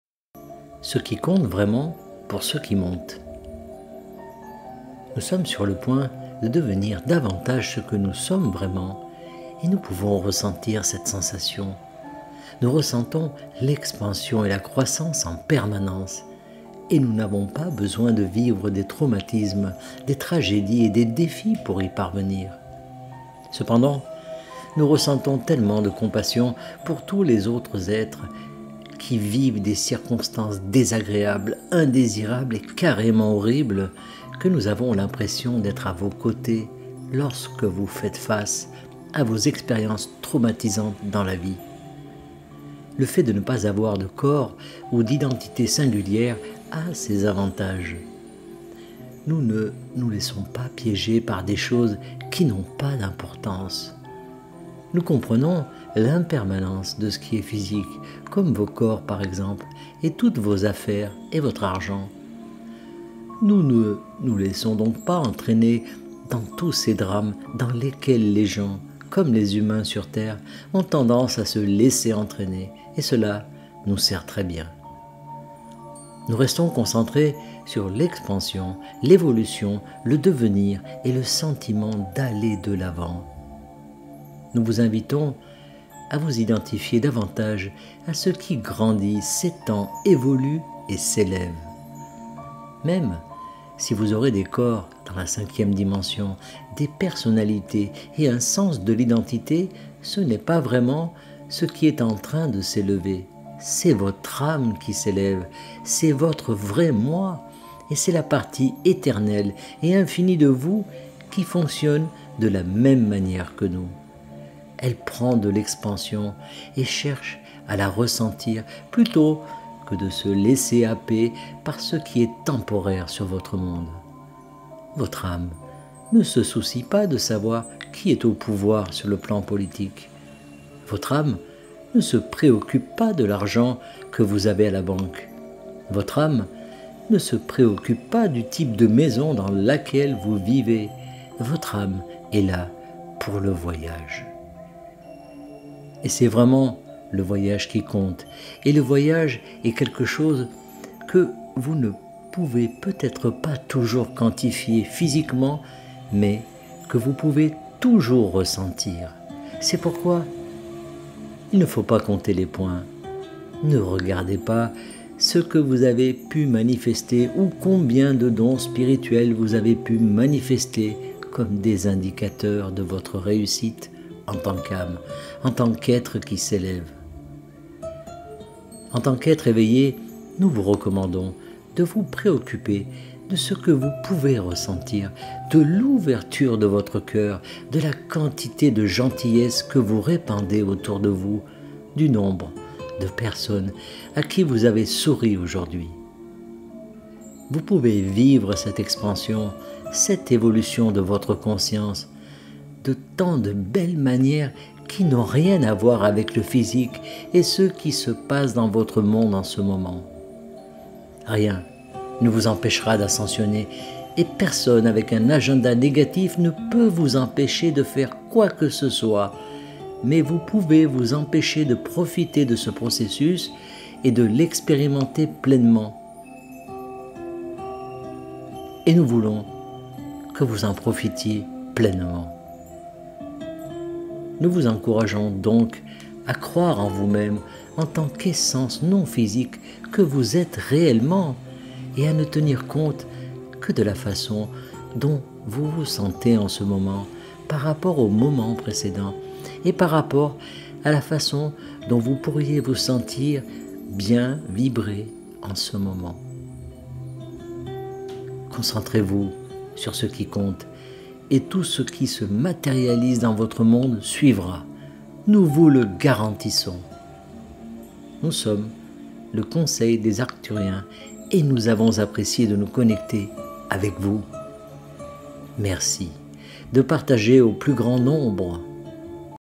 Ce qui compte vraiment pour ceux qui montent
Nous sommes sur le point de devenir davantage ce que nous sommes vraiment, et nous pouvons ressentir cette sensation. Nous ressentons l’expansion et la croissance en permanence, et nous n’avons pas besoin de vivre des traumatismes, des tragédies et des défis pour y parvenir.